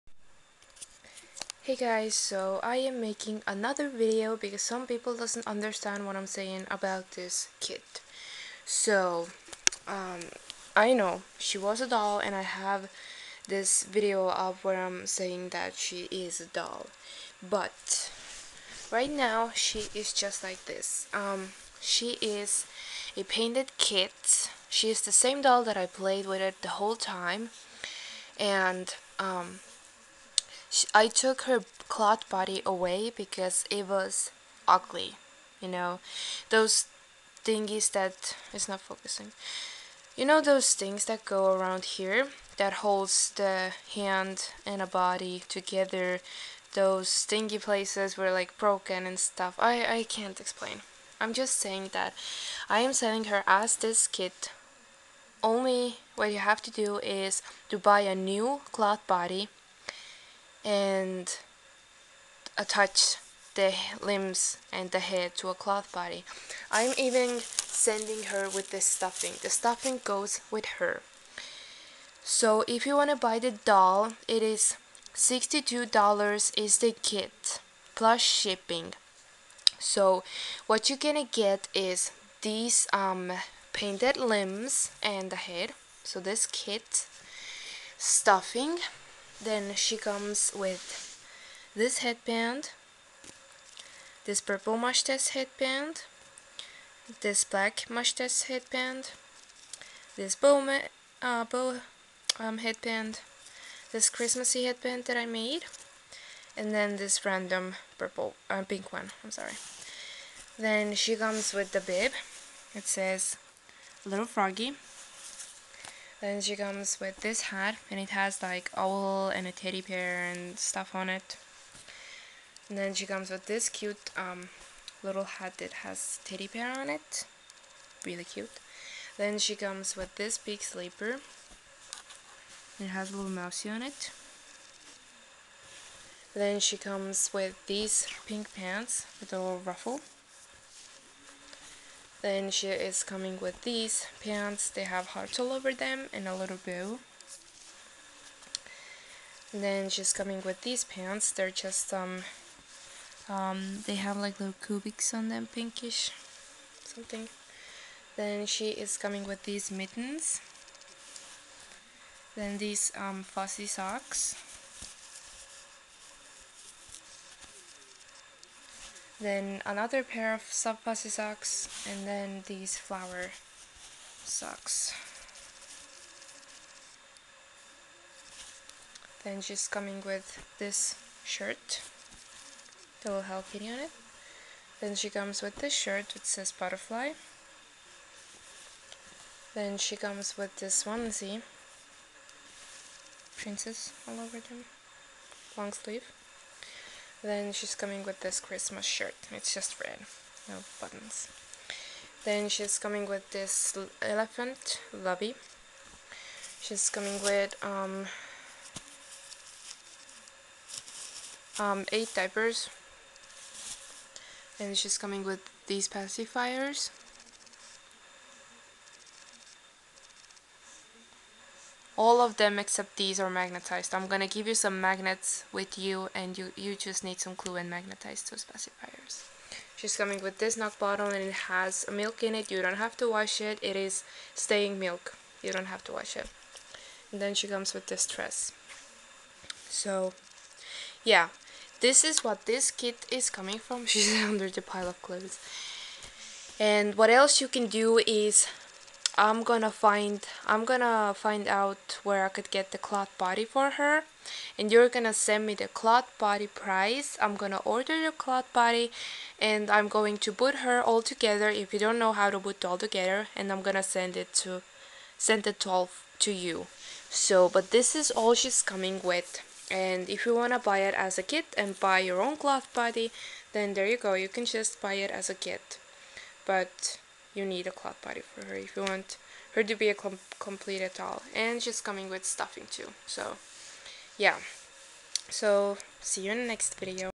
SWEET SLEEPING PEA KIT FOR SALE $62+S/H
So this is a sleeping pea kit by lura lee eagles and it ONLY a kit,not completed doll.So this video is about what she will come home with,if u are interested of her please email me and we can work something out.
NO TRADE